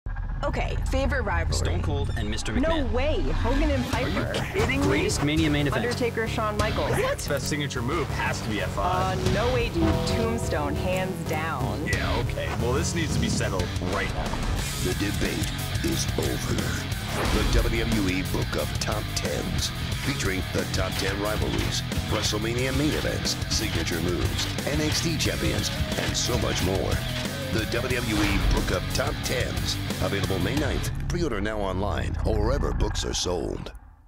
The WWE Book of Top 10s
From the greatest champions to memorable matches and everything in between, the best of WWE is ranked once and for all. Let the countdown begin!

WWE: The Book of Top 10s is packed with information and trivia and will provide hours of ammunition and controversy as fans debate the lists, arguing positioning, surprise inclusions, and snubs from 100 lists spanning five decades of sports entertainment history.

The greatest stars from WWE's past and present are featured, including Kevin Owens, AJ Styles, The New Day, Sasha Banks, John Cena, Stone Cold Steve Austin, Triple H, Randy Savage, and more!